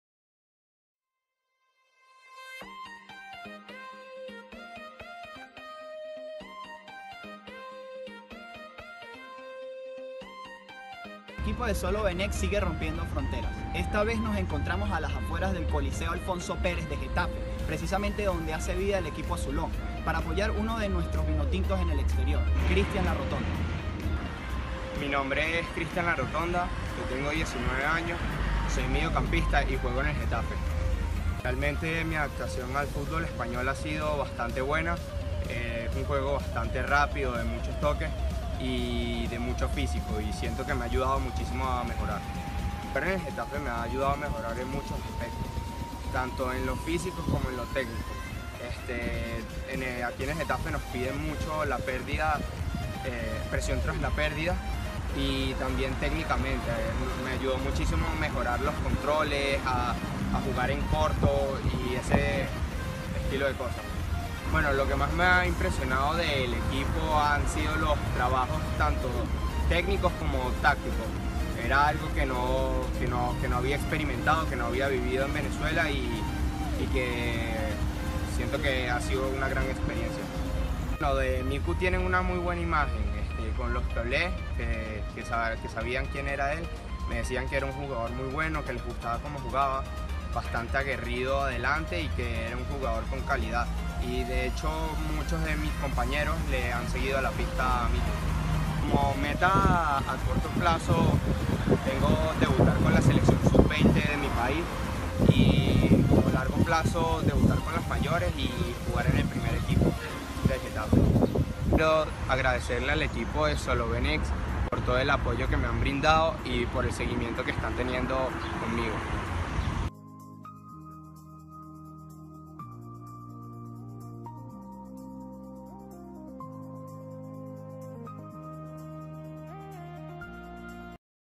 Christian Larotonda: "Me he adaptado muy bien al fútbol español"
Christian Larotonda, mediocampista venezolano que hace vida en el Juvenil A del Getafe de España, conversó con el equipo de SOLOVENEX sobre sus primeras impresiones en suelo ibérico, metas a corto, largo plazo y más.